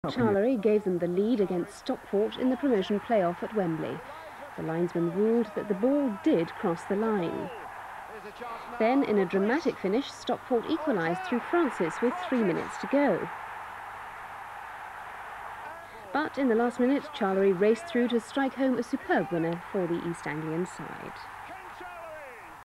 Peterborough United v Stockport County Play-off Final 1992
In my younger, care-free student days, I was a Peterborough United season ticket holder, so 24th May 1992 will forever hold fond memories for me. Posh's first ever visit to Wembley - and mine, too - culminated in the sort of finish that even the editor of Roy of the Rovers wouldn't have stood for.

That first goal (which actually wasn't) definitely crossed the line. Even though it didn't. Unless it did. But who cares? The mighty Posh won thanks to Ken Charlery's double and made it into the second tier of English football for the first time ever. What a day out that was!

This is a news report I recorded that very evening. Can you believe that in those days, play-off finals weren't broadcast live? The biggest achievement in Posh's history reduced to less than 30 seconds on the local news...

(Please remember that this is an off-air VHS recording, so don't go expecting 1080p HD quality. I'm fairly certain that this video isn't commercially available, and I've uploaded it for its historic interest only.